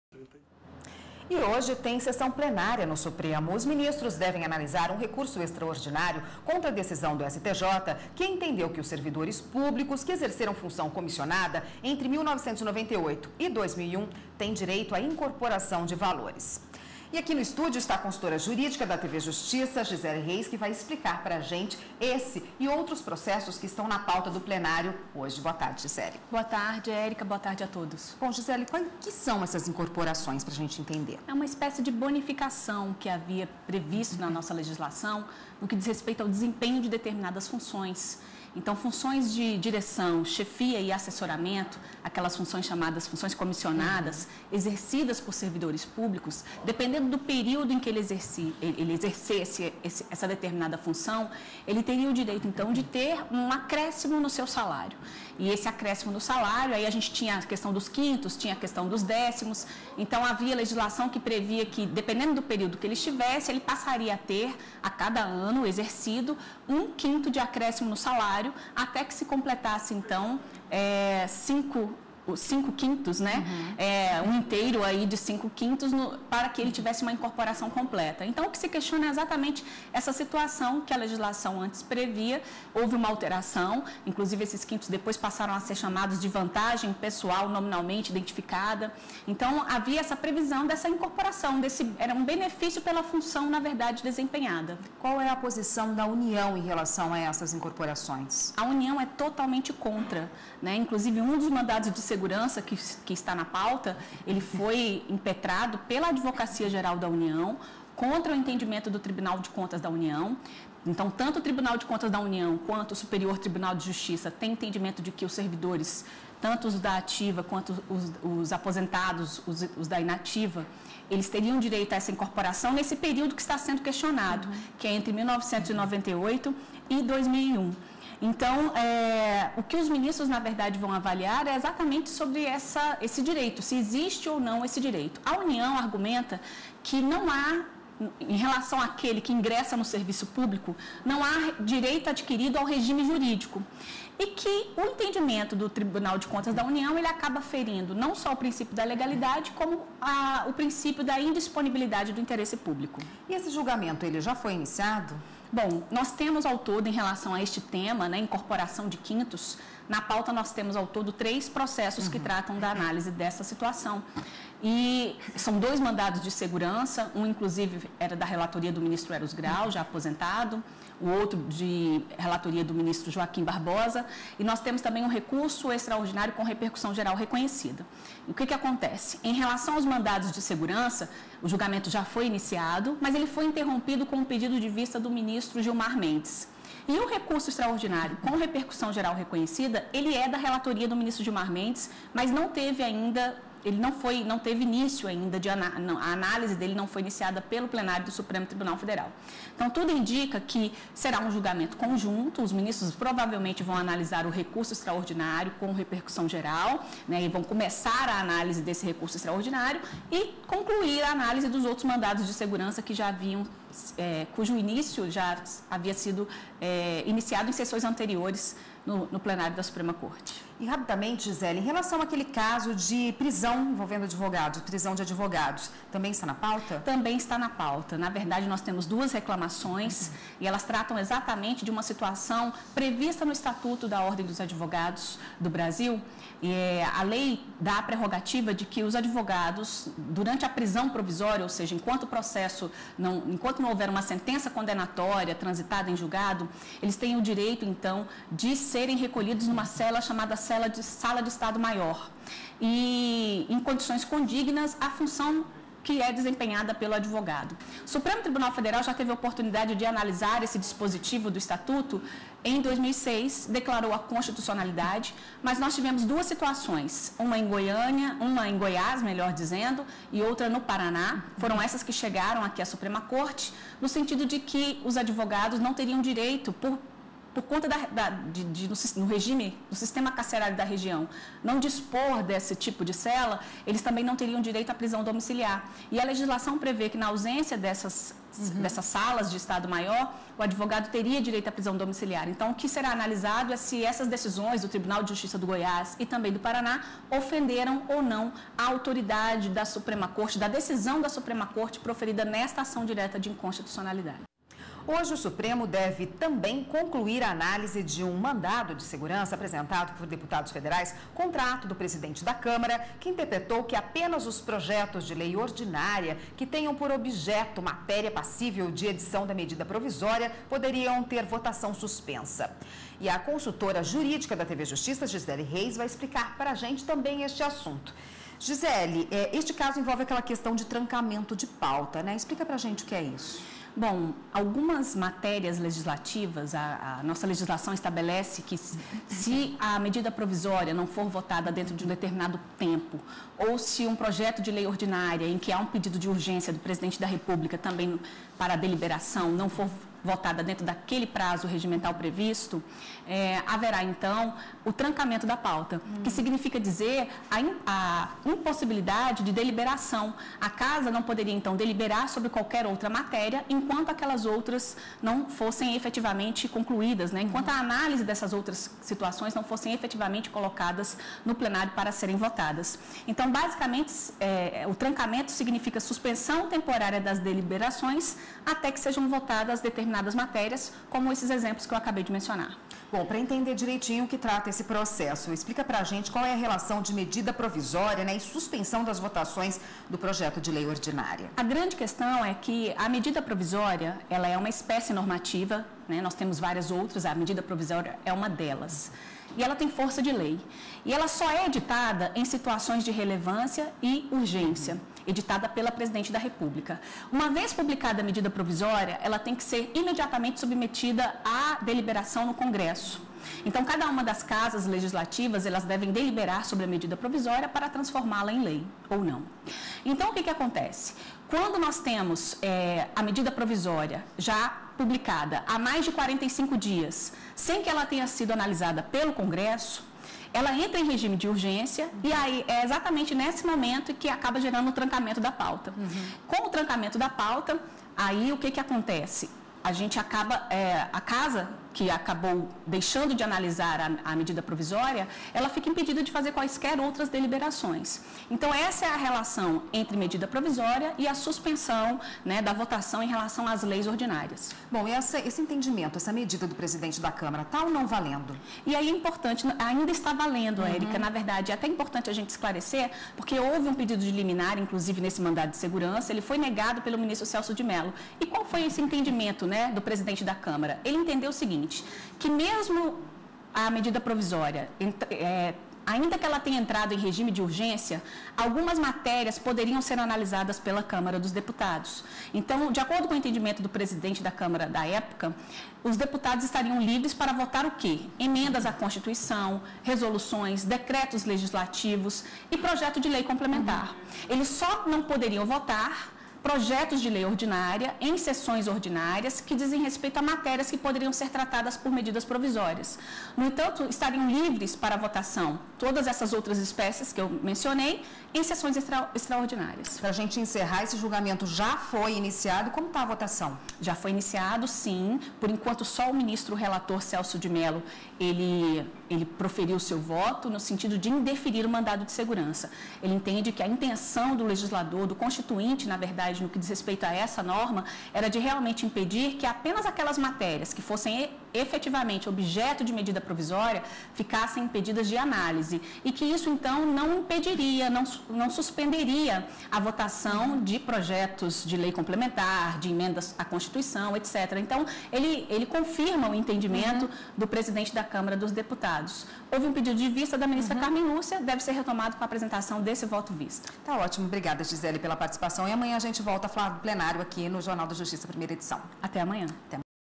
Sessão Plenária STF (18/03/15)
Hoje (18) tem sessão plenária no Supremo Tribunal Federal. Os ministros devem  analisar um recurso extraordinário contra decisão do STJ que entendeu que servidores públicos que exerceram função comissionada entre 1998 e 2001 têm direito à incorporação de valores. Hoje, o STF deve também concluir a análise de  um mandado de segurança apresentado por deputados federais contra ato do presidente da Câmara que interpretou que apenas os projetos de lei ordinária ,que tenham por objeto matéria passível de edição de medida provisória, poderiam ter a votação suspensa.